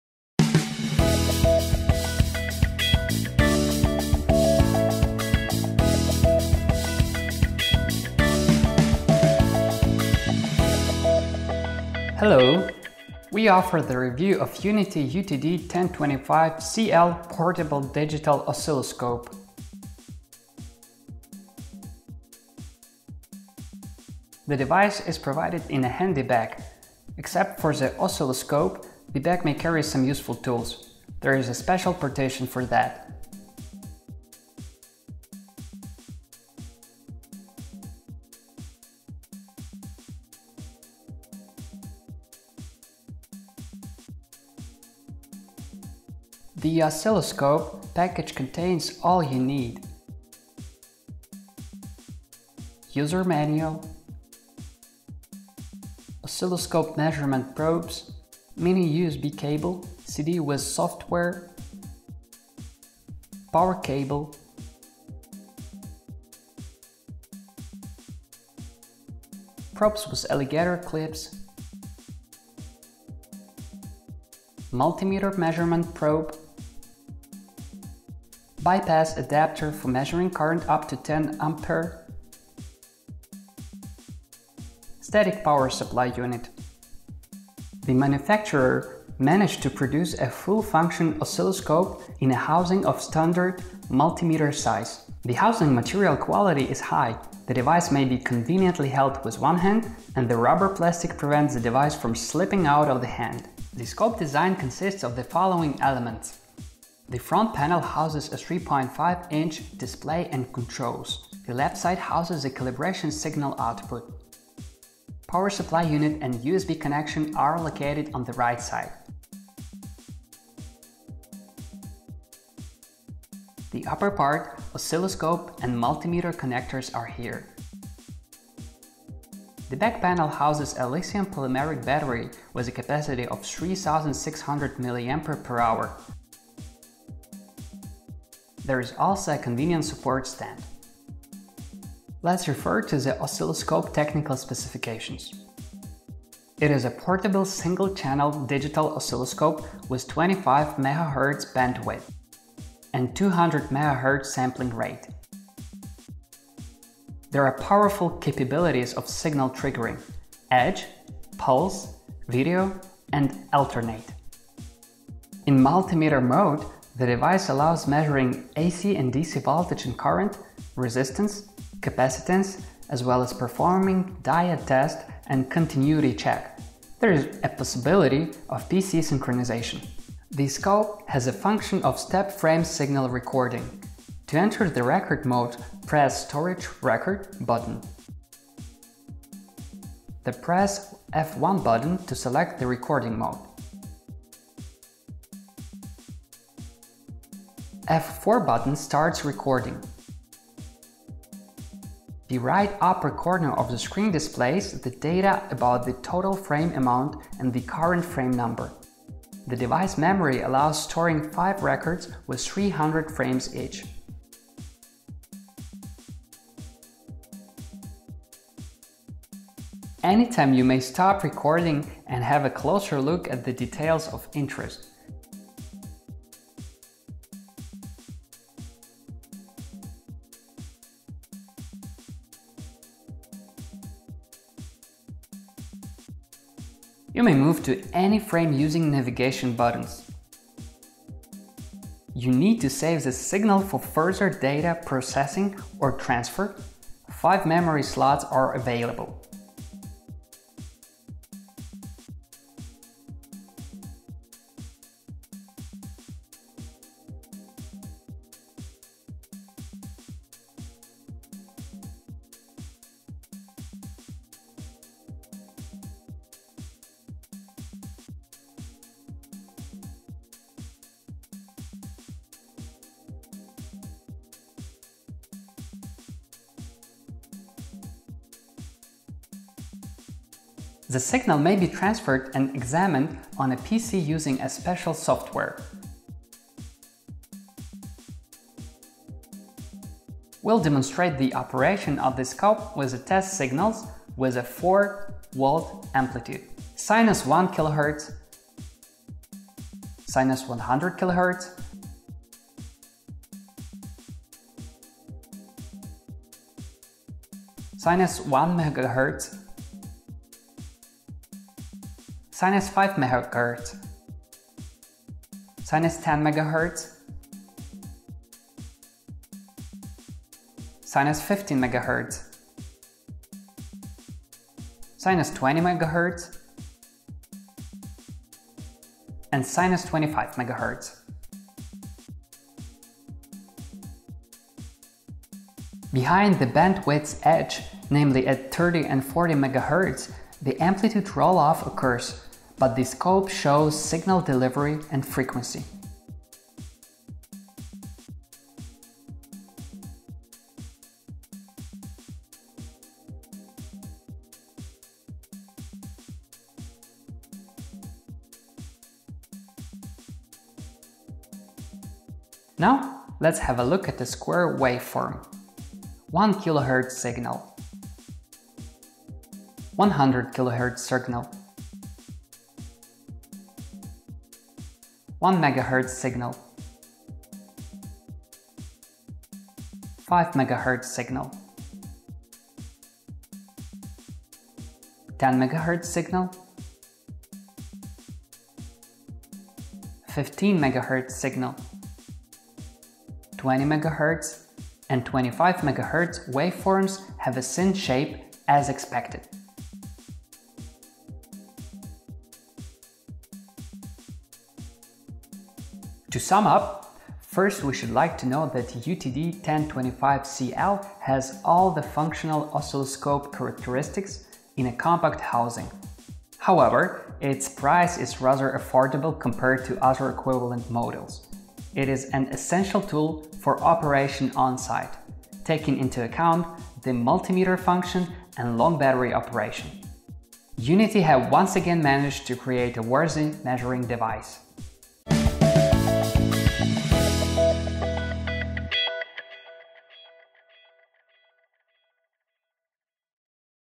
UNI-T UTD1025CL Handheld Digital Oscilloscope
UNI-T UTD1025CL is a lightweight single-channel handheld scopemeter with a bandwidth of 25 MHz and a sample rate of 200 MS/s. The UTD1025CL may also perform multimeter measurements: AC/DC voltage and current, resistance, capacity, diode and continuity tests.

Let’s take a closer look at the design of the oscilloscope. The front panel has a 3.5-inch display and controls. The left side contains a calibration signal output. A power supply unit and an USB connection are located on the right side, oscilloscope and multimeter connectors – on top. The back panel houses a lithium-polymeric battery with a capacity of 3600 mAh. Thanks to the high-quality housing material, the oscilloscope may be conveniently held with one hand and the rubber plastic prevents the device from slipping out. A convenient support stand completes the design.

In terms of technical specifications, the UTD1025CL, it is a portable single-channel digital oscilloscope, offering powerful capabilities of signal triggering: Edge, Pulse, Video, and Alternate.
In the multimeter mode, the device allows measuring AC and DC voltage and current, resistance, and capacitance and may run diode tests and continuity checks. The UTD1025CL may be synchronized with a PC.
The oscilloscope has a function of step-frame signal recording. To enter the record mode, press STORAGE/RECORD button, then press F1 button to select the recording mode. F4 button starts recording. The top right corner of the screen displays the data about the total frame number and the number of the current frame. The in-built memory allows storing up to 5 records with 300 frames in each.
The user may stop recording and take a closer look at specific data at any point. You may move to any frame using navigation buttons. Save the signal for further data processing or transfer with 5 available memory slots.
This signal may be transferred and examined on a PC using special-purpose software.
We'll demonstrate the operation of the oscilloscope with test signals with an amplitude of 4 V: sin 1 kHz; sin 100 kHz; sin 1MHz; sin 5 MHz; sin 10 MHz; sin 15 MHz; sin 20 MHz; and sin 25 MHz.
Despite amplitude roll-off beyond the bandwidth edge, specifically, at 30 and 40 MHz, the device displays signal delivery and frequency.
Waveforms of 1 kHz signal; 100 kHz signal; 1 MHz signal; 5 MHz signal; 10 MHz signal; 15 MHz signal; 20 MHz, and 25 MHz at a square waveform have a sin shape as expected.

The device is sold in a handy bag. In addition to the oscilloscope itself, you can accommodate a number of useful items in a special compartment of the bag.
The manufacturer succeeded in delivering a full-function oscilloscope in a housing of standard multimeter size.

The oscilloscope package contains pretty much everything you need:
✓ oscilloscope measurement probes
✓ mini USB cable
✓ power cable
✓ probes with alligator clips
✓ multimeter measurement probe
✓ bypass adapter for measuring current up to 10 A
✓ static power supply unit
✓ CD with software

To sum up, UTD1025CL has all the functional features of an oscilloscope in a compact housing. However, its price is more affordable compared to other similar models. Thanks to the multimeter features and a long battery run time, it’s an essential tool for field operations.
UNI-T have once again succeeded in creating a high-performance affordable measuring device.